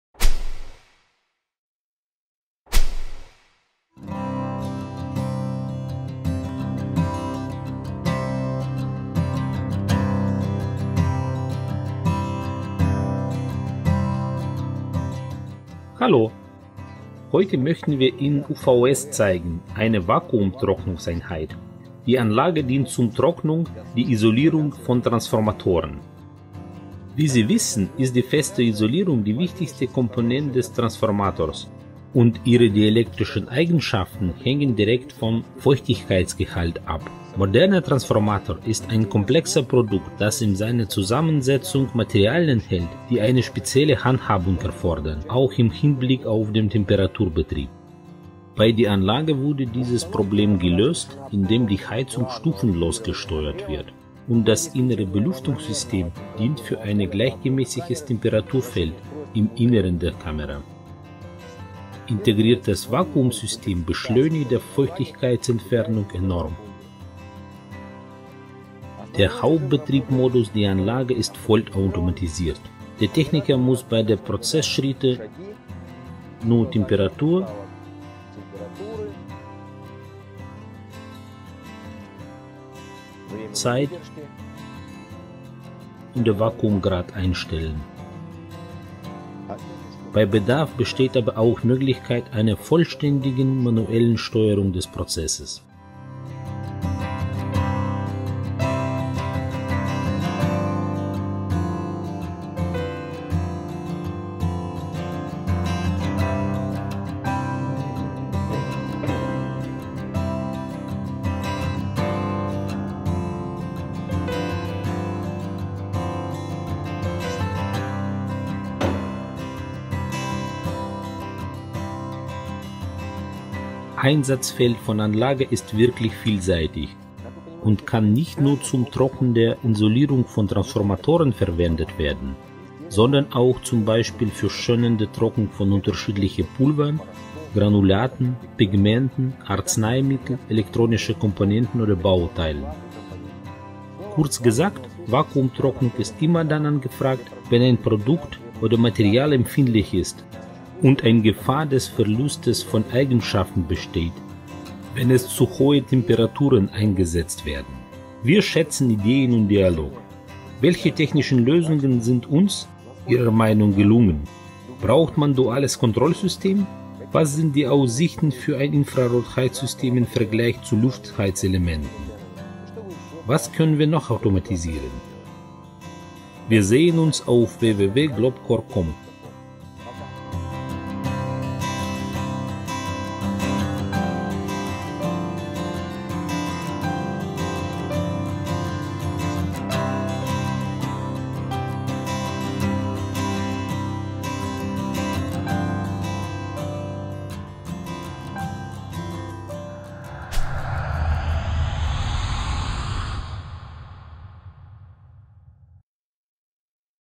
Transformator Vakuumofen
Mehr sehen Wir stellen Ihnen den Transformator-Vakuumofen UVS-6S vor, der für die Trocknung der Feststoffisolierung von Leistungstransformatoren bestimmt ist.

Eine übliche Praxis bei der Herstellung oder Reparatur von Leistungstransformatoren besteht darin, die Feuchtigkeit aus der Papierisolierung zu entfernen. Die getrocknete Isolierung hat eine dielektrische Stärke, die 20mal größer als die nicht getrocknete ist, da Feuchtigkeit die dielektrische Durchschlagsfestigkeit negativ beeinflusst.

Abhängig von der Art der Transformatoren gibt es mehrere Möglichkeiten, Feuchtigkeit aus dem aktiven Teil zu entfernen. So kann man z.B. diese mit einem Vakuumofen entfernen.

Die Trocknung von Transformatoren mit Vakuumöfen hat folgende Vorteile:

- intensivere Verdunstung der Feuchtigkeit im Vergleich zur Trocknung mit warmer Luft oder natürliche Trocknung;
- optimale Gesamtleistung und reduzierte Verarbeitungszeit

GlobeCore bietet einen Transformator-Vakuumofen mit Modifikationen an, die an die spezifischen Bedürfnisse des Kunden angepasst sind. Das Design berücksichtigt Produktivität, Abmessungen und Platzierung, Ladeverfahren, Automatisierungsgrad usw.